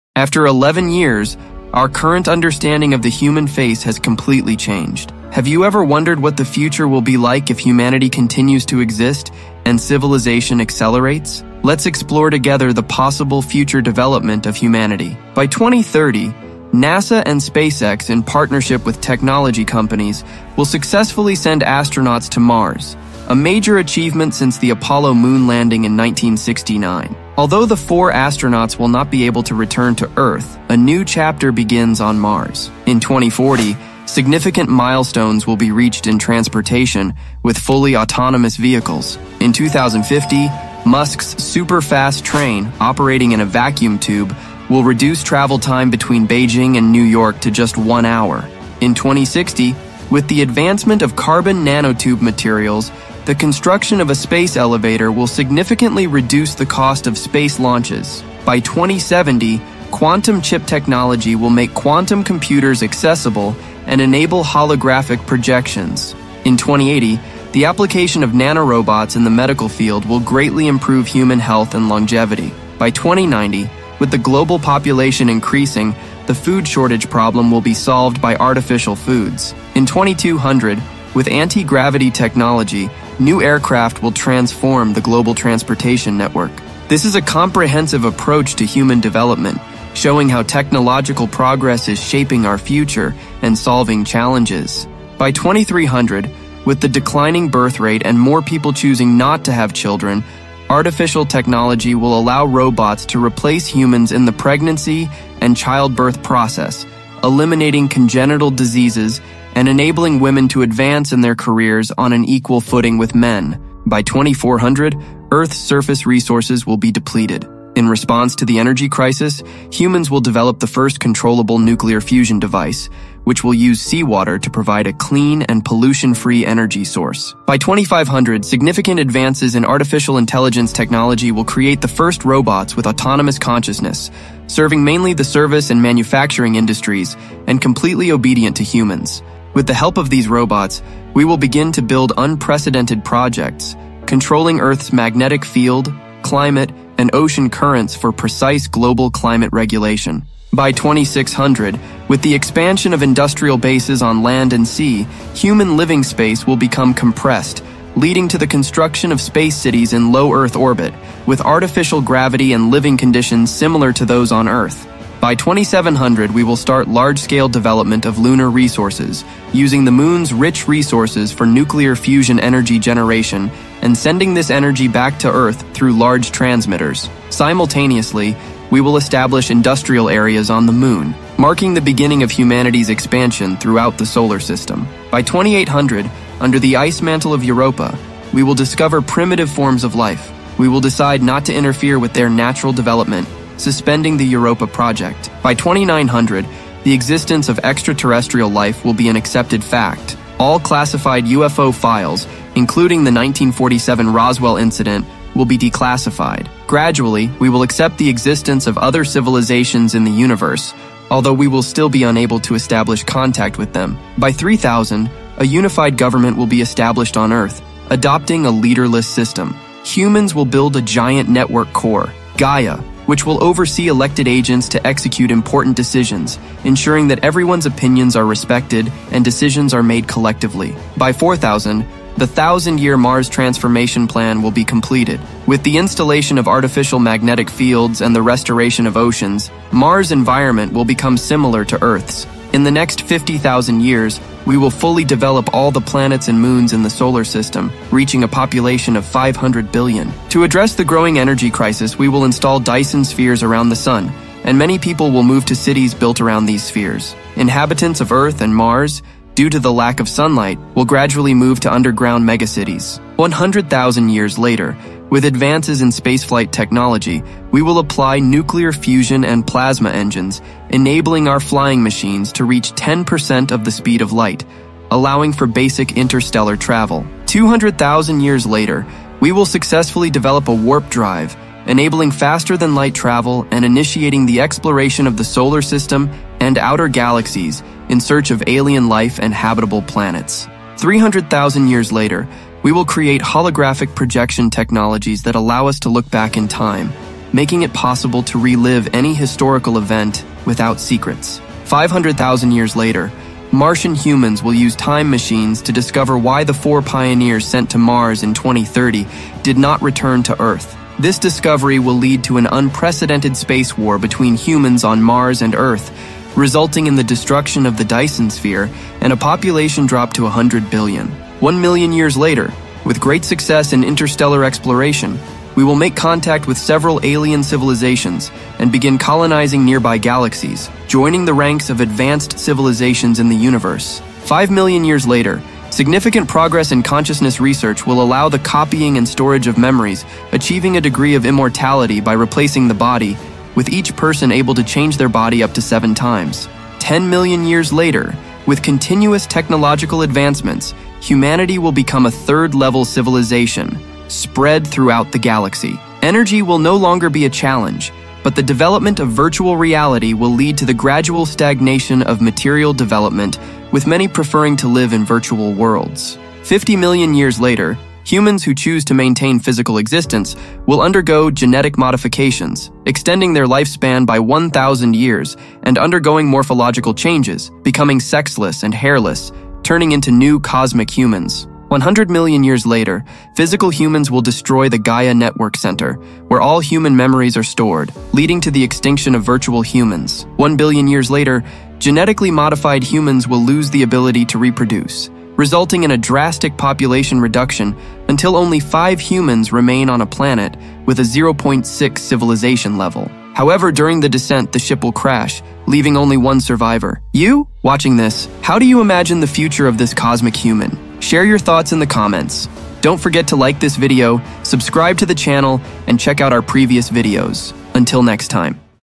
INSANE: What the human race will be like in a billion years may be beyond our imagination!
Explore the extraordinary future of humanity! Dive into future predictions and groundbreaking advancements, from Mars colonization with SpaceX and NASA to autonomous vehicles and Elon Musk’s Hyperloop. Discover how quantum computing, nanorobots, and artificial intelligence will transform our world.

Join us as we blend scientific foresight with imaginative possibilities. Until next time friends!